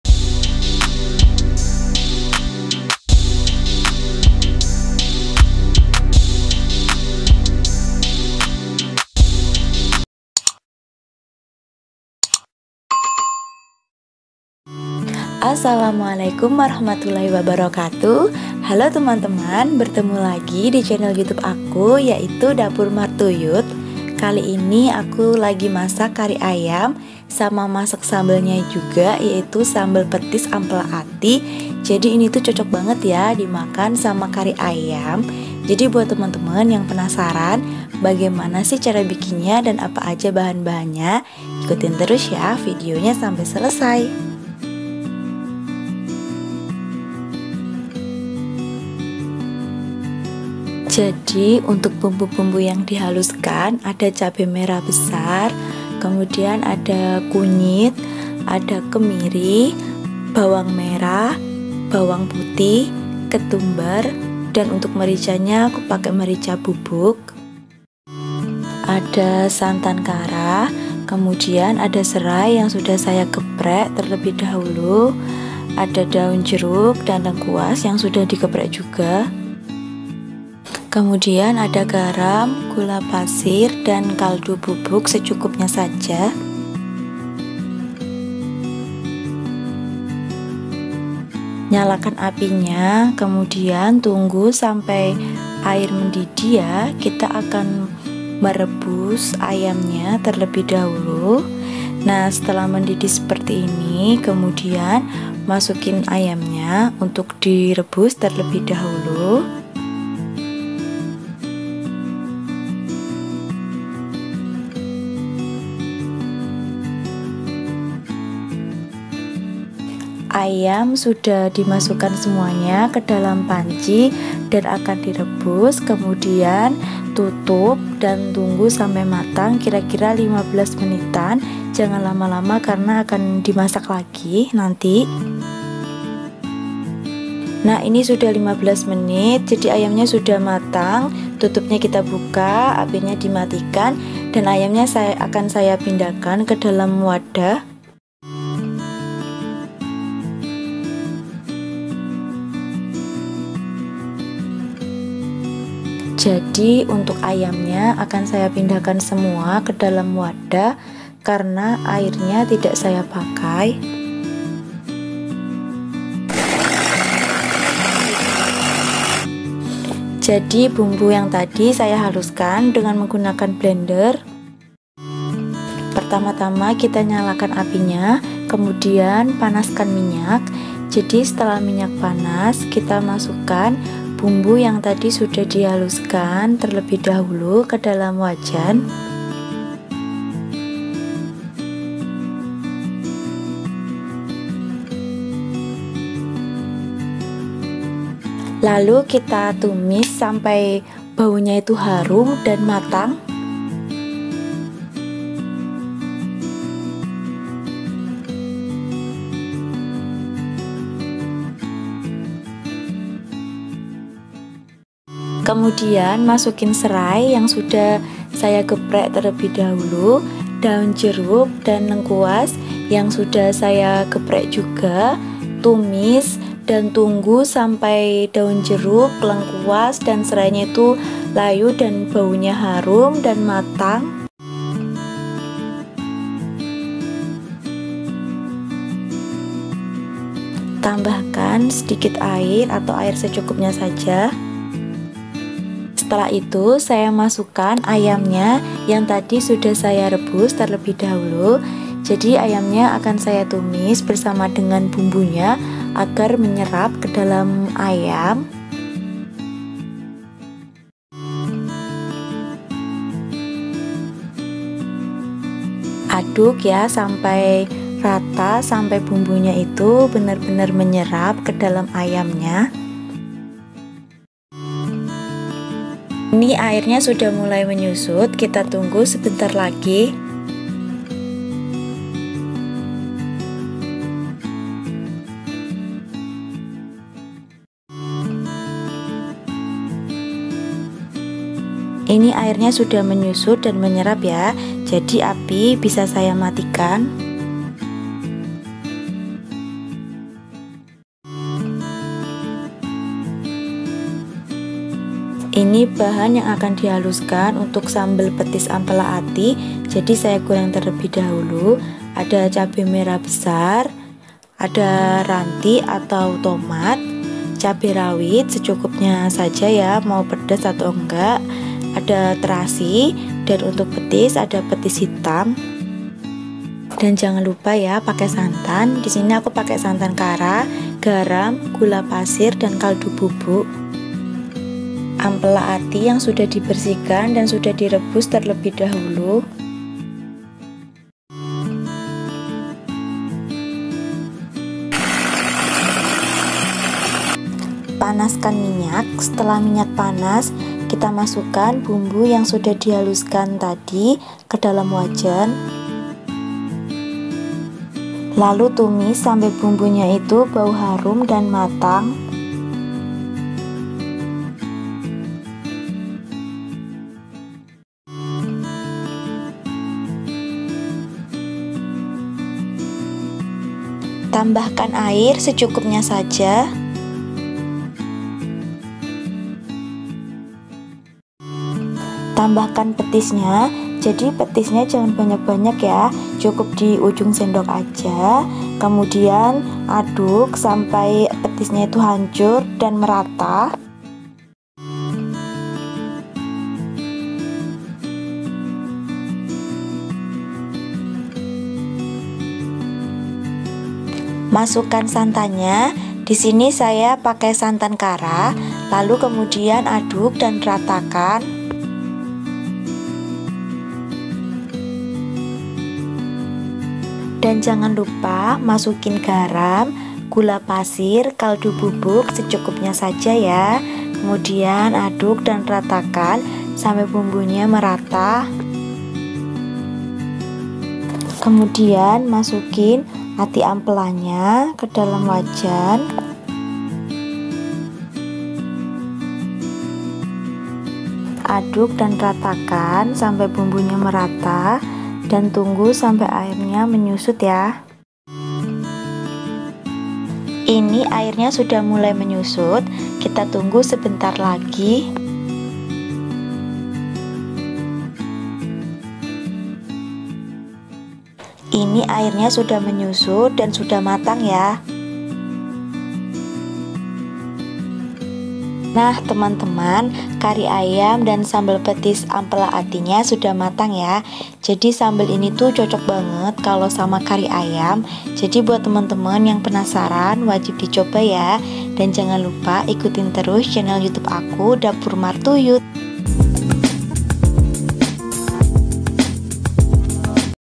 Kari ayam sambal petis ampela ati... wajib di coba...!!! enak & simpel banget
Kari ayam cocok banget kalau di makannya sama sambel petis ampela ati, enak banget.

Bahan2 kari ayam:
Daging ayam
Bawang merah
Bawang putih
Cabe merah besar
Kunyit
Ketumbar
Kemiri
Merica
Santan kara
Kaldu bubuk
Garam
Gula pasir
Serai
Daun jeruk
Lengkuas

Bahan2 sambel petis ati ampela:
Ampela ati ayam
Cabe merah besar
Cabe rawit
Tomat/Ranti
Terasi
Santan kara
Kaldu bubuk
Gula pasir
Garam